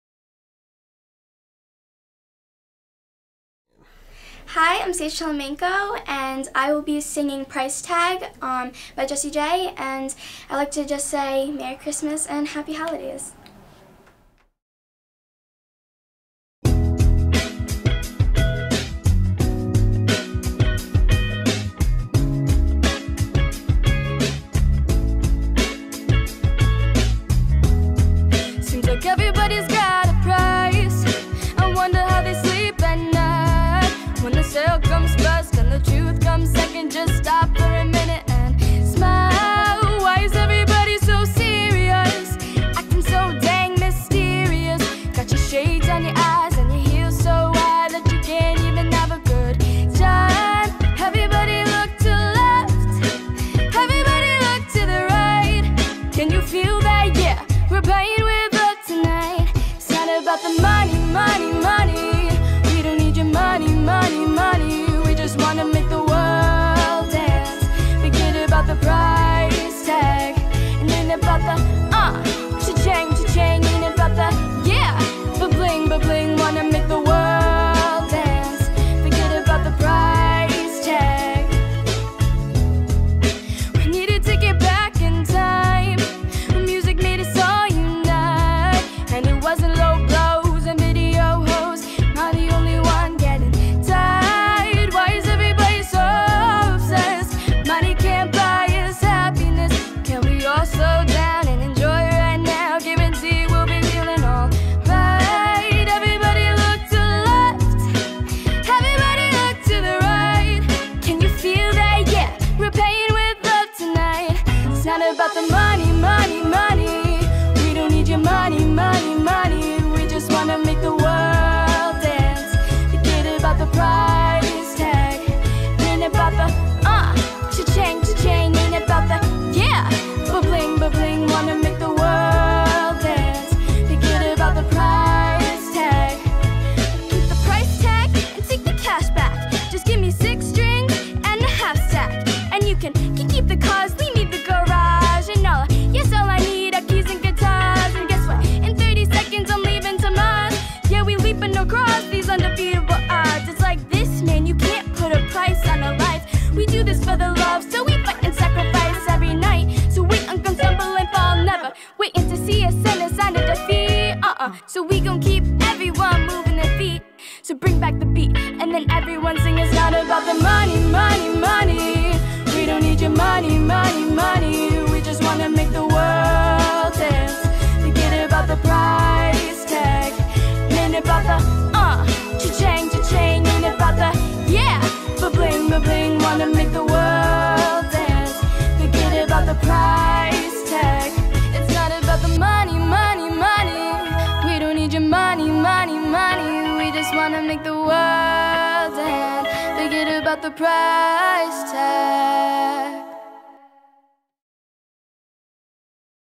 Sage T. live from DEA Records Recording Studio - Holiday Performance 2020
Cover by Sage T.
Instructor: Julia Freitor